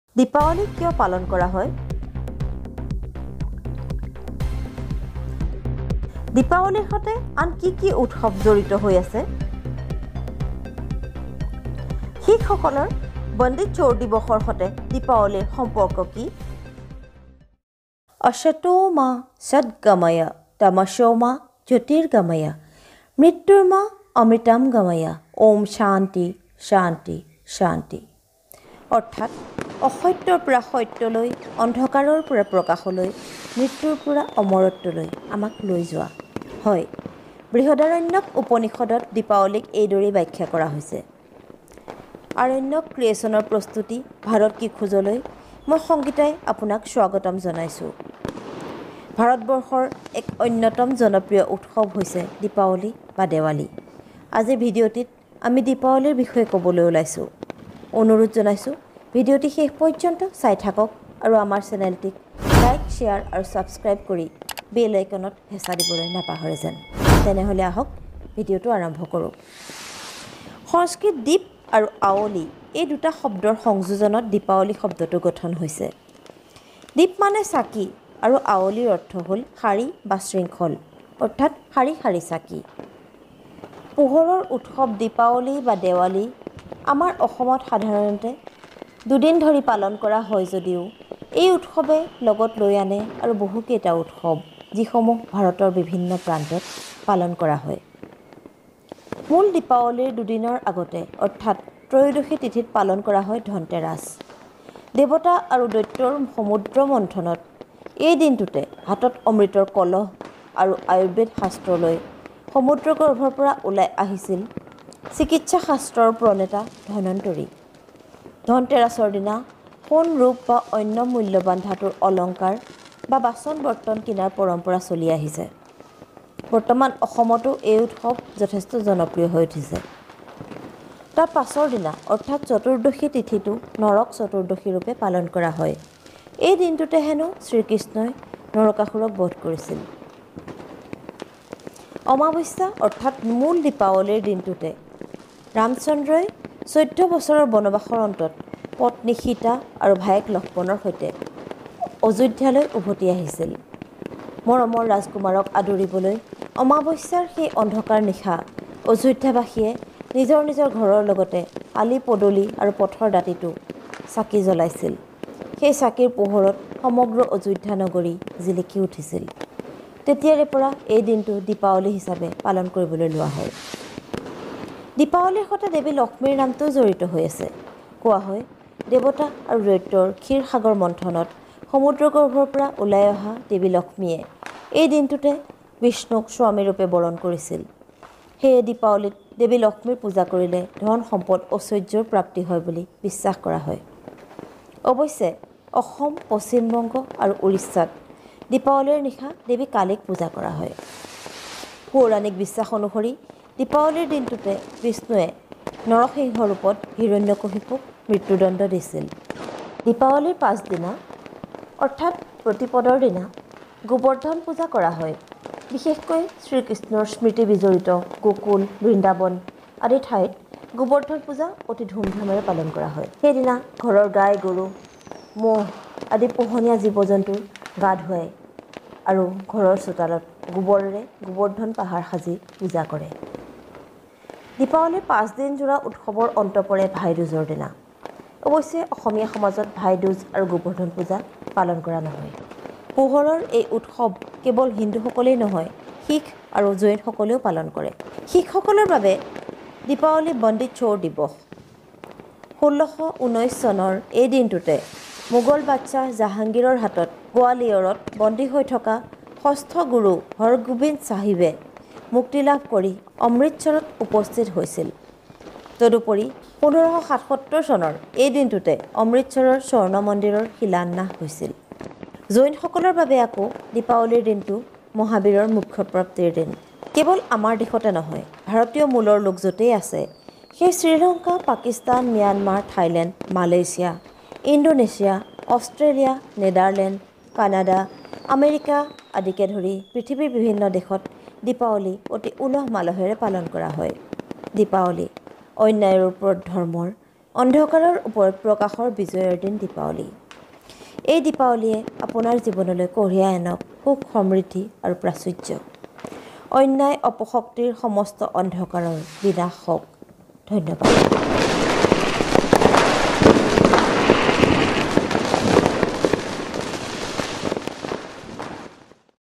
সংগীতা ভৰালীৰ প্ৰাচ্য-চিন্তা/ Sangeeta Bharali's Eastern thought.
দীপাৱলীৰ সতে জড়িত হৈ আছে আৰু বহুকেইটা উৎসৱ। দীপাৱলীৰ দুদিনৰ আগতে, অৰ্থাৎ ত্ৰয়োদশী তিথিত পালন কৰা হয় ধনতেৰাছ। চতুৰ্দশীৰ দিনটো নৰক চতুৰ্দশী বুলি কোৱা হয়। সেই দিনটোতে হেনো শ্ৰীকৃষ্ণই নৰকাসুৰক বধ কৰিছিল। মূল দীপাৱলীৰ দিনটোতে ৰামচন্দ্ৰই চৈধ্য বছৰৰ বনবাস সম্পূৰ্ণ কৰি পত্নী সীতা আৰু ভায়েক লক্ষ্মণক লগত লৈ অযোধ্যালৈ  উভতি আহিছিল। পৌৰাণিক বিশ্বাস অনুসৰি সমুদ্ৰ মন্থনত ওলাই অহা দেৱী লক্ষ্মীয়ে এই দিনটোতে বিষ্ণুক স্বামীৰূপে বৰণ কৰিছিল আৰু  বিষ্ণুৱে নৰসিংহ ৰূপত হিৰণ্যকশিপুক বধ কৰাৰ দিনটোও আছিল দীপাৱলী।